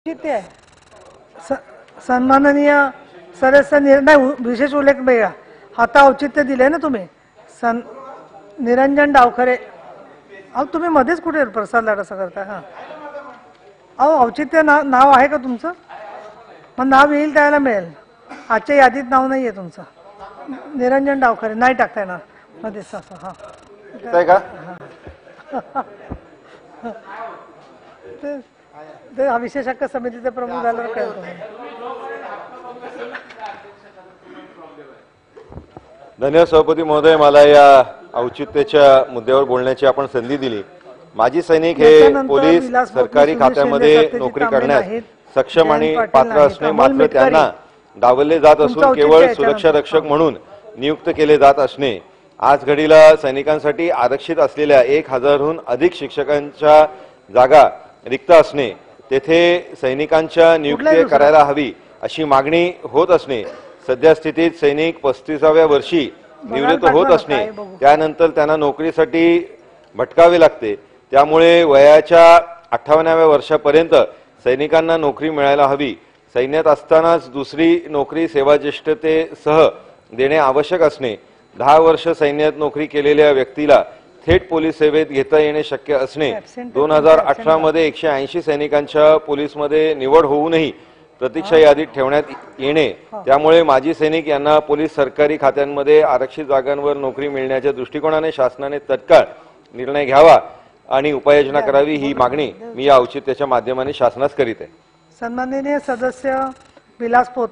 MLC Niranjan Davkhare - Budget Session 2023
राज्य सरकारच्या विविध विभागांत सेवेसाठी इच्छूक असताना सैनिकांना केवळ सुरक्षारक्षकाची नोकरी दिली जात असल्याबद्दल विधान परिषदेचे लक्ष वेधले.